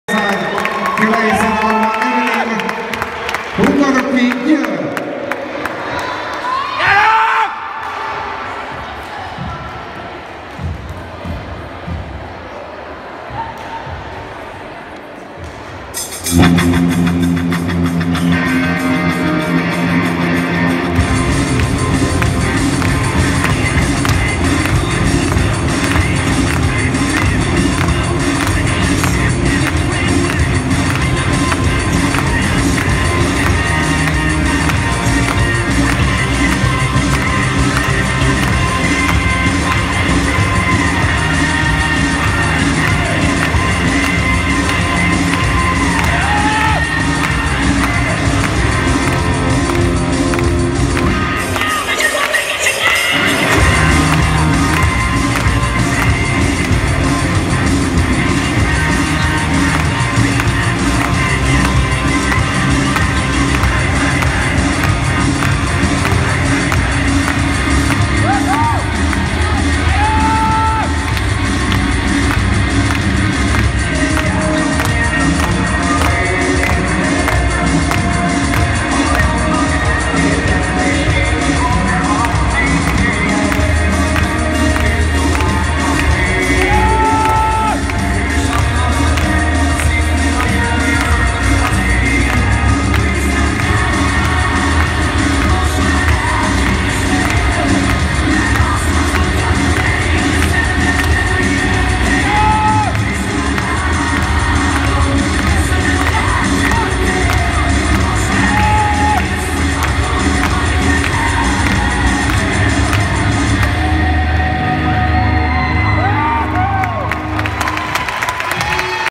HUNGARockY Te Fodor Zsigmond és Kleizer Anna
Nyugat-Magyarországi Területi bajnokság Ikrény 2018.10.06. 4. Helyezés